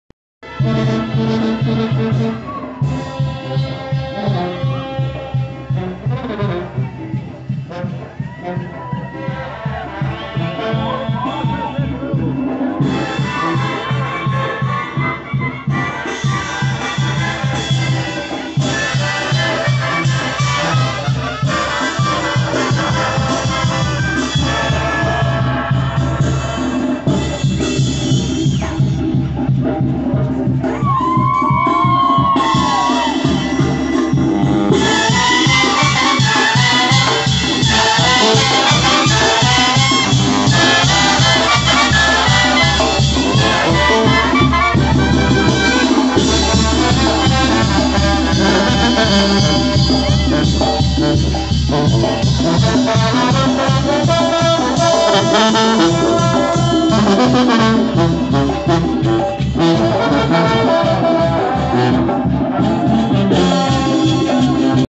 Quince años en mi pueblo.
Así se festejan unos quince años en mi pueblo, después de la celebración religiosa, la comida y el baile en este jardín.